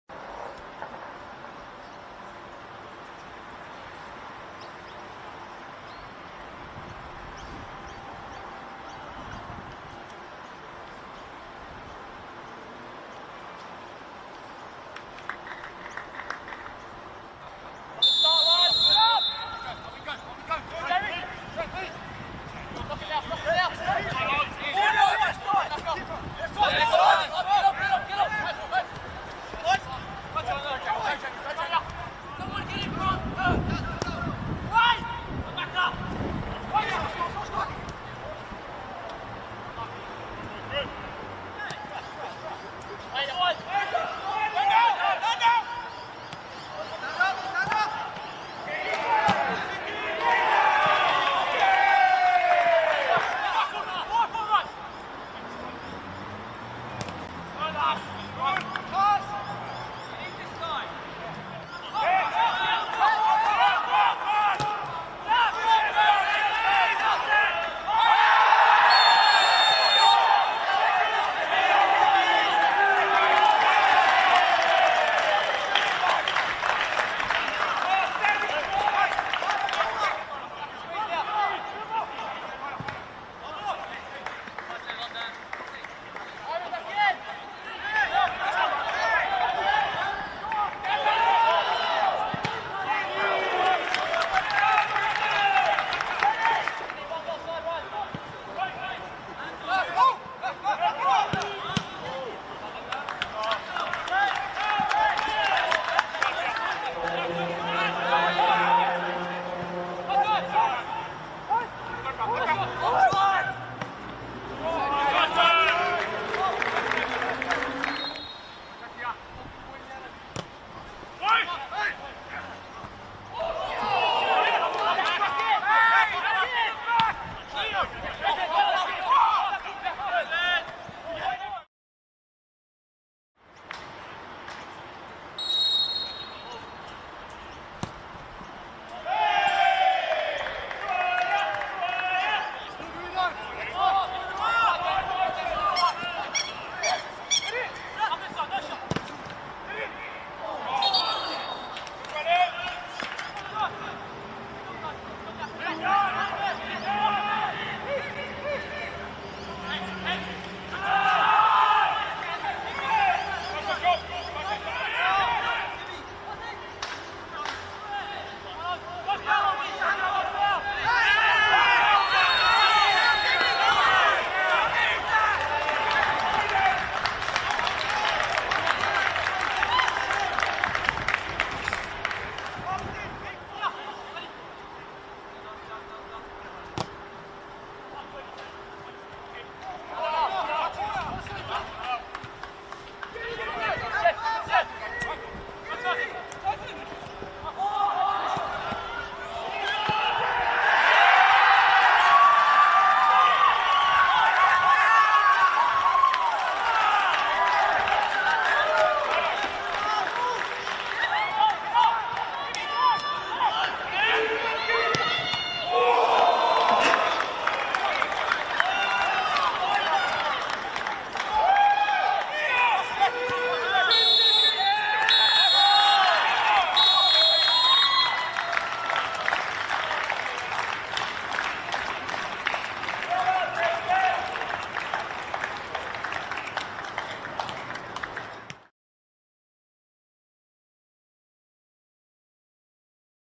NPL R17 Highlights: Brisbane Roar vs Gold Coast Knights
NPL Queensland Men 2024 – Round 17: Brisbane Roar v Gold Coast Knights
Perry Park, Bowen Hills
Saturday, 24 July

Brisbane Roar – 0
Gold Coast Knights – 1 (Bradden INMAN 86’)

🎥 Football Queensland